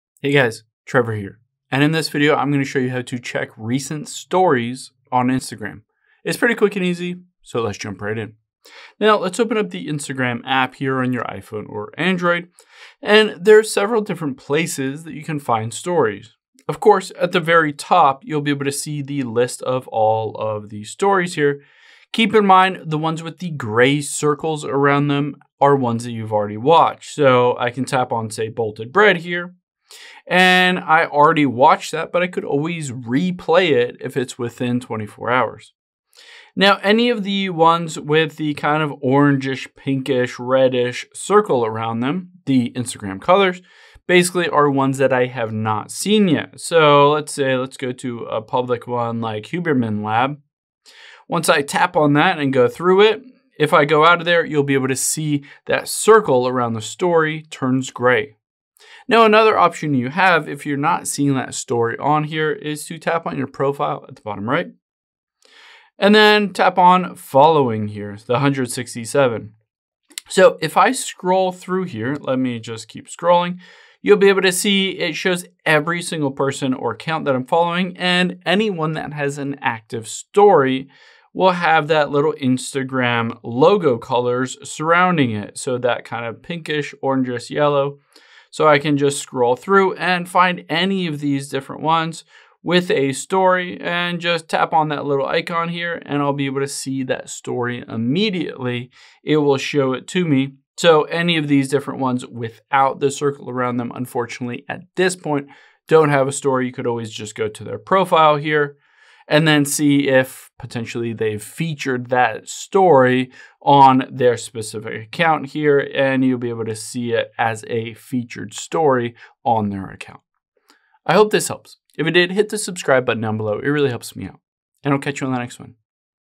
How To Check Recent Stories On Instagram
Here's how to check your recent Instagram stories if you want to rewatch any, save them, send messages, etc.